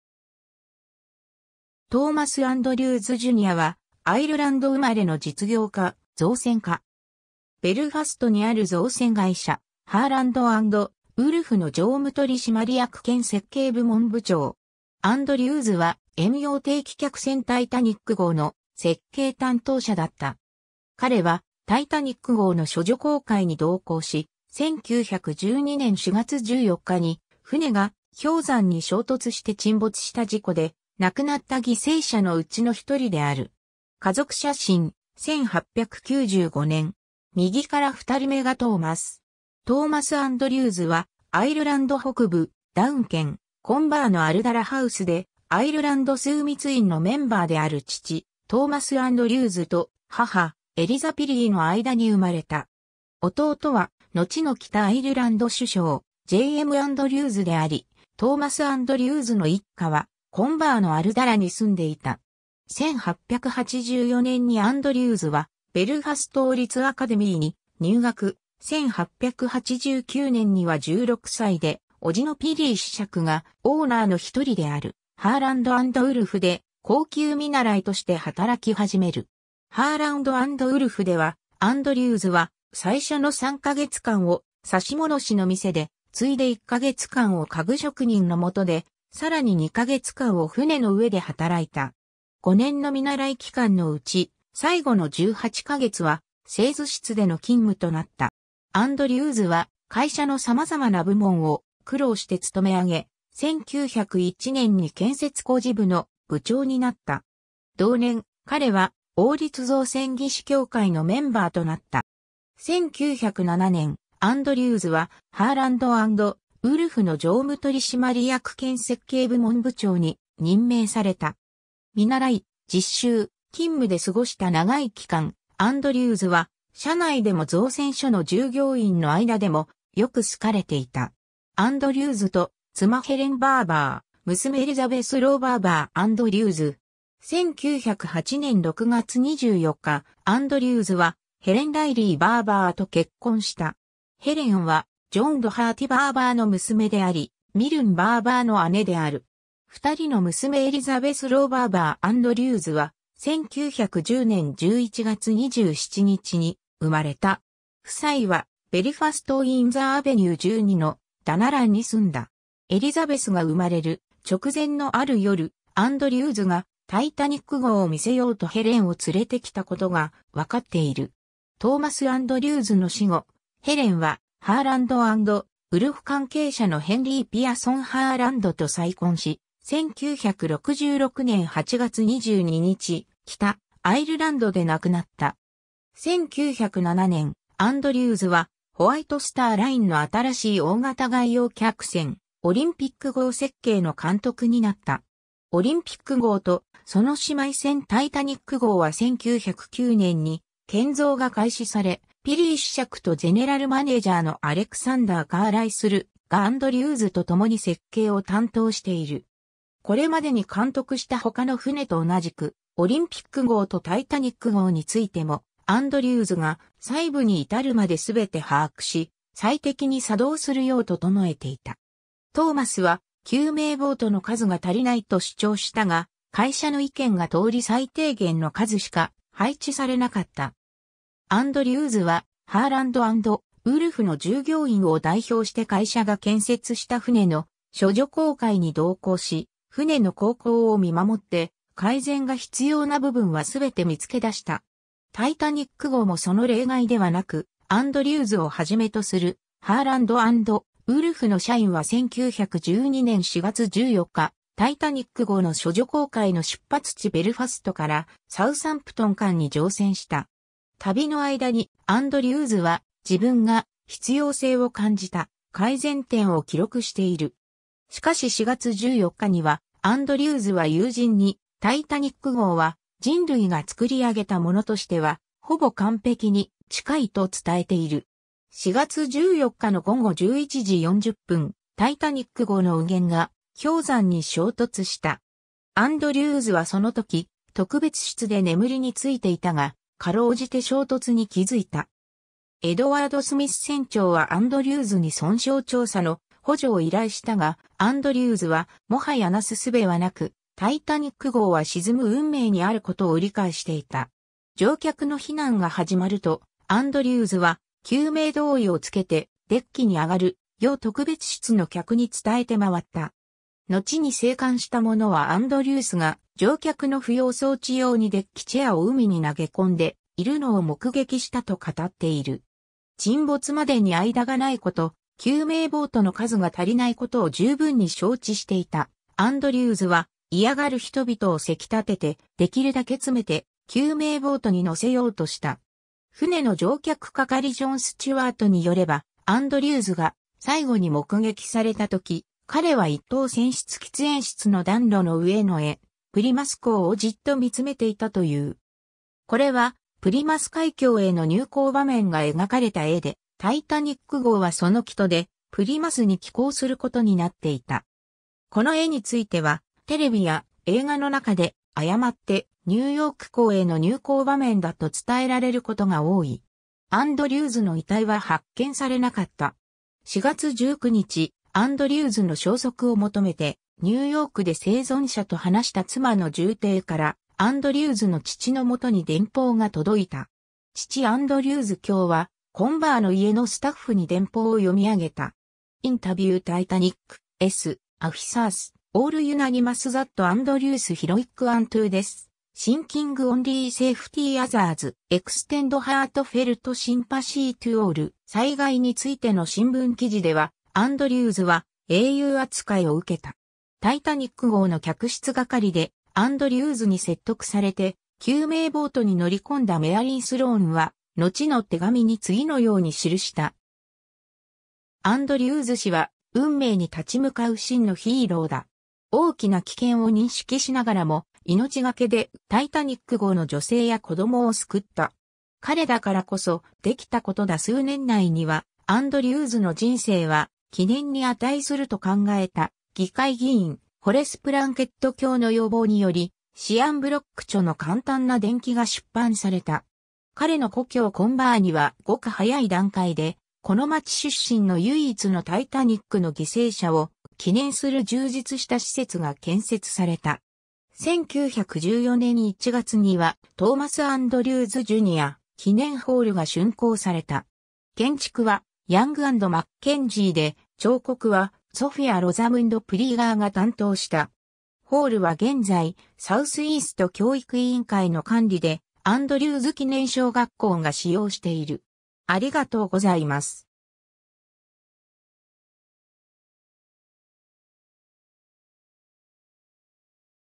トーマス・アンドリューズ (造船家)
トーマス・アンドリューズ・ジュニア（Thomas Andrews, Jr., 1873年2月7日 - 1912年4月15日）は、アイルランド生まれの実業家・造船家。
ベルファストにある造船会社ハーランド・アンド・ウルフの常務取締役兼設計部門部長。
アンドリューズは遠洋定期客船タイタニック号の設計担当者だった。
彼はタイタニック号の処女航海に同行し、1912年4月14日に船が氷山に衝突して沈没した事故で、亡くなった犠牲者のうちの1人である。
 家族写真、1895年。
右から2人めがトーマス。
 トーマス・アンドリューズは、アイルランド北部、ダウン県コンバーのアルダラ・ハウスで、アイルランド枢密院のメンバーである父トーマス・アンドリューズと母エリザ・ピリーの間に生まれた。
 弟はのちの北アイルランド首相J.M.アンドリューズであり、トーマス・アンドリューズの一家はコンバーのアルダラに住んでいた。
1884年にアンドリューズはベルファスト王立アカデミーに入学、1889年には16歳で、伯父のピリー子爵がオーナーの一人であるハーランド・アンド・ウルフで、高級見習いとして働き始める。
 ハーランド・アンド・ウルフでは、アンドリューズは最初の3ヶ月間を指物師の店で、次いで1ヶ月間を家具職人の元で、さらに2ヶ月間を船の上で働いた。
5年の見習い期間のうち、最後の18カ月は、製図室での勤務となった。
アンドリューズは会社の様々な部門を苦労して勤めあげ、1901年に建設工事部の部長になった。
同年、彼は王立造船技師協会のメンバーとなった。
1907年、アンドリューズはハーランド・アンド・ウルフの常務取締役兼設計部門部長に任命された。
見習い、実習、勤務で過ごした長い期間、アンドリューズは、社内でも造船所の従業員の間でも、よく好かれていた。
 アンドリューズと妻ヘレン・バーヴァー、娘エリザベス・ロウ・バーヴァー・アンドリューズ。
 1908年6月24日、アンドリューズはヘレン・ライリー・バーヴァーと結婚した。
ヘレンは、ジョン・ドハーティ・バーヴァーの娘であり、ミルン・バーヴァーの姉である。
2人の娘エリザベス・ロウ・バーヴァー・アンドリューズ（イニシャル ELBA ）は、1910年11月27日に生まれた。
夫妻はベルファストウィンザー・アベニュー 12 の「Dunallan」に住んだ。
 エリザベスが生まれる直前のある夜、アンドリューズが、タイタニック号を見せようとヘレンを連れてきたことがわかっている。
トーマス・アンドリューズの死後、ヘレンはハーランド・アンド・ウルフ関係者のヘンリー・ピアソン・ハーランドと再婚し、1966年8月22日、北アイルランドで亡くなった。
 1907年、アンドリューズは、ホワイト・スター・ラインの新しい大型外洋客船、オリンピック号設計の監督になった。
オリンピック号と、その姉妹船タイタニック号は1909年に建造が開始され、ピリー子爵とジェネラルマネージャーのアレクサンダー・カーライスルがアンドリューズとともに設計を担当している。
これまでに監督した他の船と同じく、オリンピック号とタイタニック号についても、アンドリューズが細部に至るまですべて把握し、最適に作動するよう整えていた。
トーマスは救命ボートの数が足りないと主張したが会社の意見が通り最低限の数しか配置されなかった。
 アンドリューズは、ハーランド・アンド・ウルフの従業員を代表して会社が建設した船の処女航海に同行し、船の航行を見守って、改善が必要な部分はすべて見つけ出した。
タイタニック号もその例外ではなく、アンドリューズをはじめとするハーランド・アンド・ウルフの社員は1912年4月14日、タイタニック号の処女航海の出発地ベルファストからサウサンプトン間に乗船した。
旅の間に、アンドリューズは自分が必要性を感じた改善点を記録している。
しかし4月14日には、アンドリューズは友人に「タイタニック号は人類が作り上げたものとしては、ほぼ完璧に近い」と伝えている。
 4月14日の午後11：40 、タイタニック号の右舷が氷山に衝突した。
アンドリューズはその時、特別室で眠りに就いていたが、かろうじて衝突に気付いた。
エドワード・スミス船長はアンドリュ...